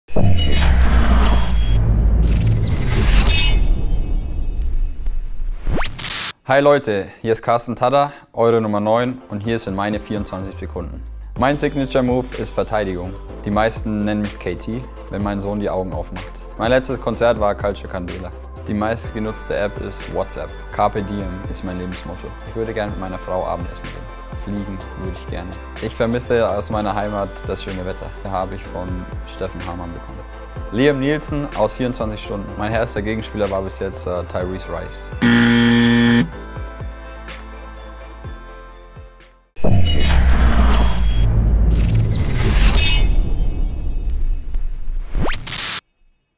24 Sekunden: Karsten Tadda
Die Shotclock läuft herunter und Ihr bekommt in einem Fragen-Stakkato einige neue Informationen über unsere Spieler. Heute: Unser Verteidigungsminister Karsten Tadda!